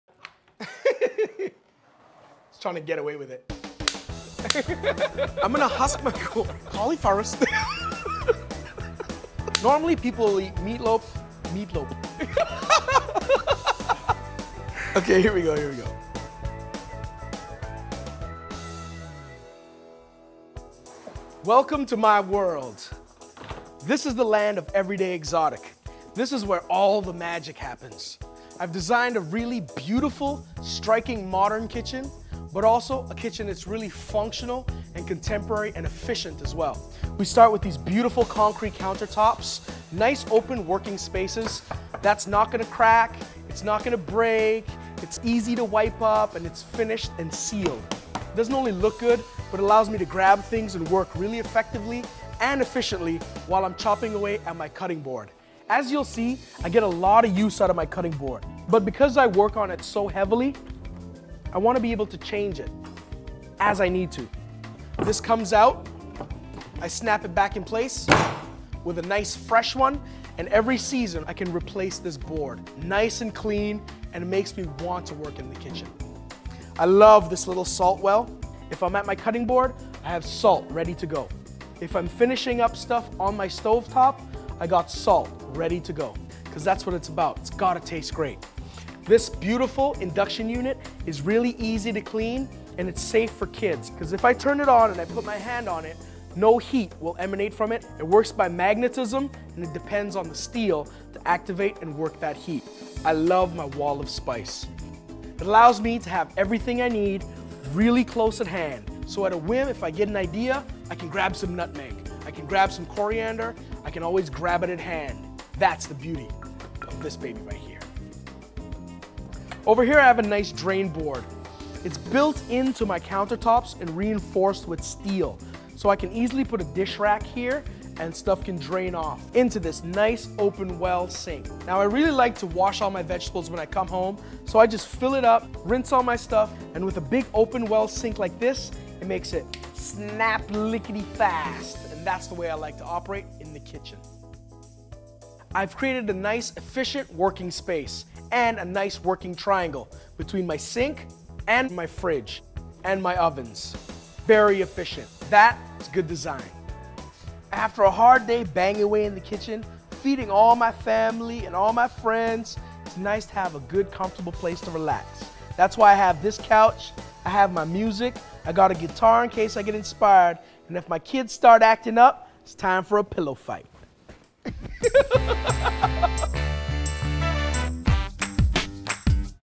SET Tour Master-H.264 Lg.mov
"Everyday Exotic" Set Tour - Behind the Design of Host Roger Mooking's set Kitchen.